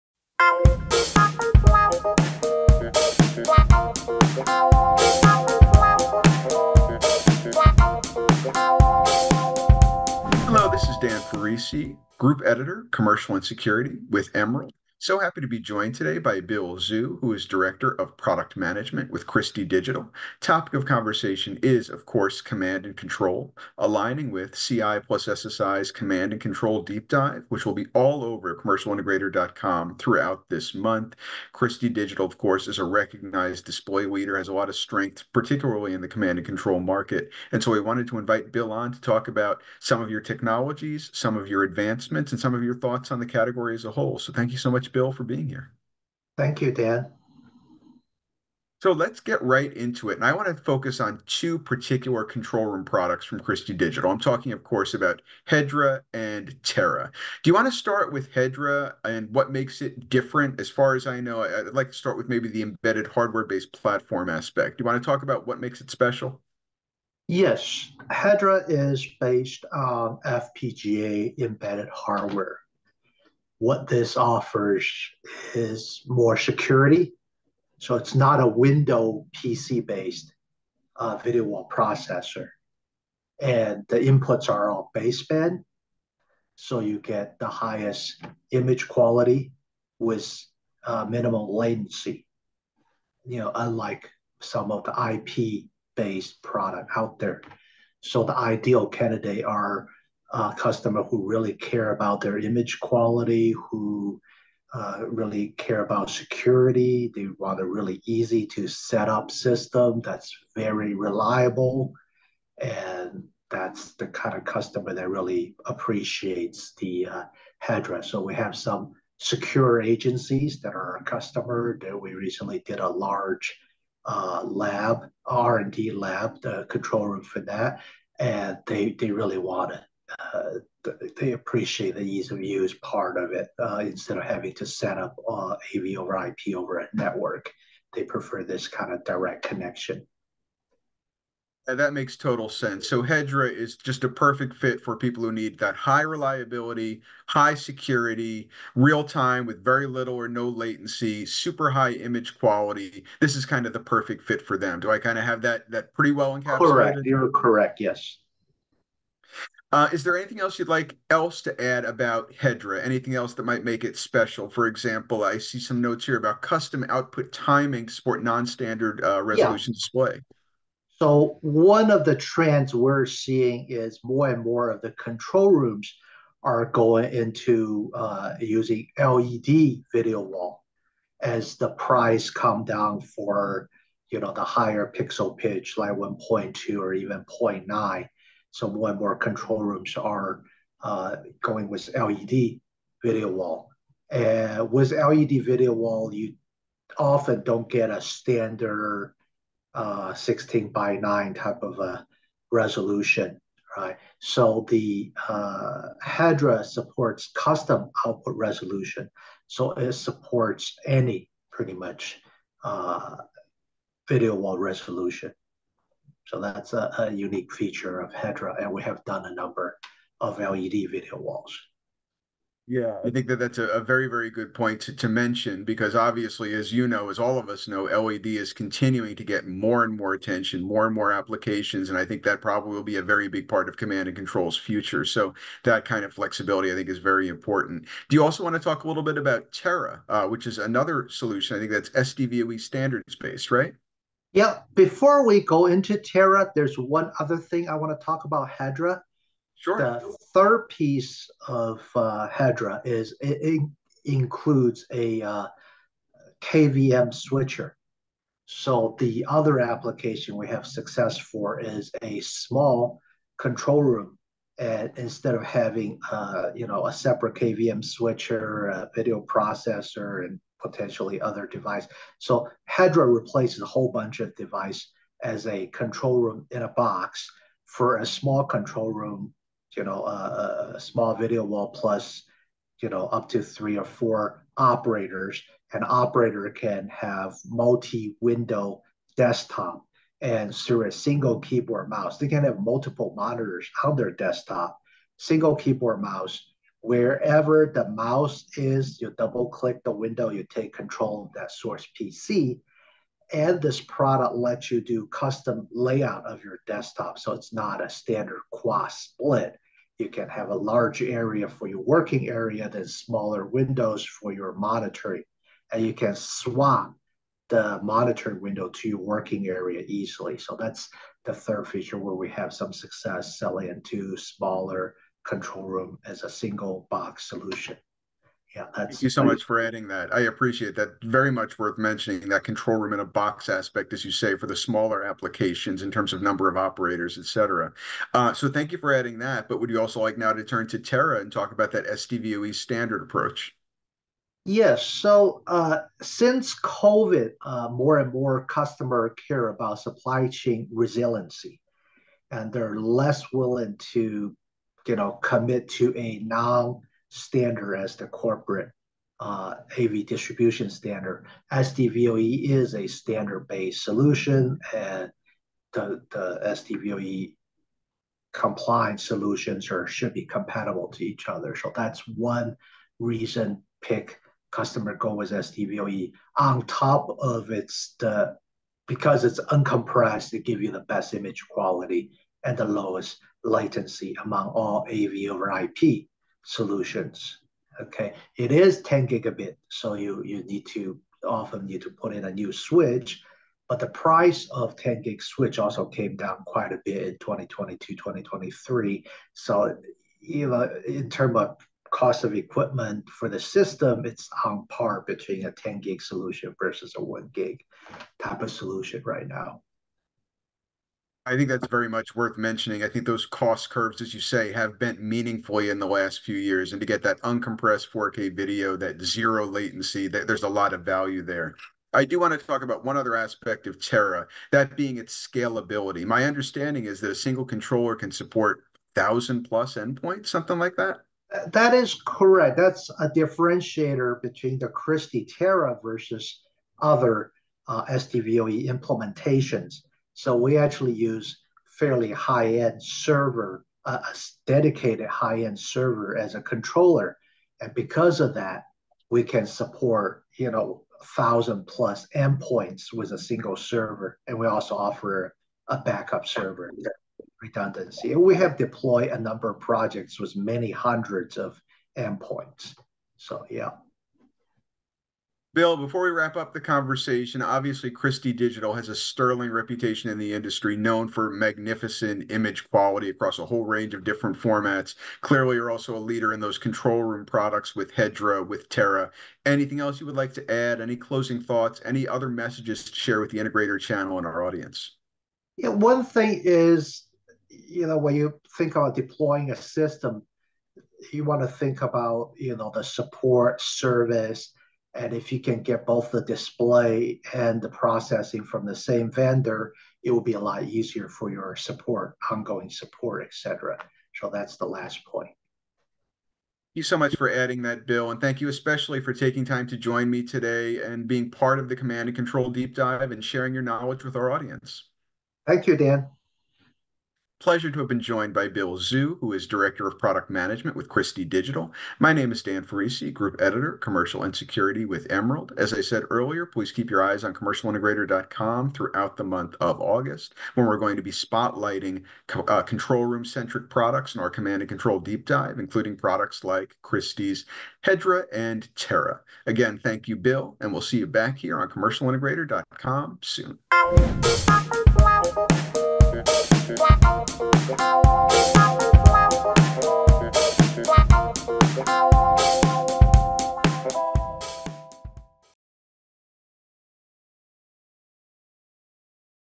Christie Shares Command-and-Control Solutions: Hedra and Terra
In this exclusive interview, Christie Digital spotlights its leading command-and-control solutions: Hedra and Terra. Bill Zhu, director of product management at Christie, offers detailed insights into the features and benefits of these solutions. Christie Hedra, for example, is a videowall processor with built-in KVM functionality that's well suited to small and medium-sized videowall processing. Christie Terra, meanwhile, is an SDVoE standard-based AV-over-IP solution.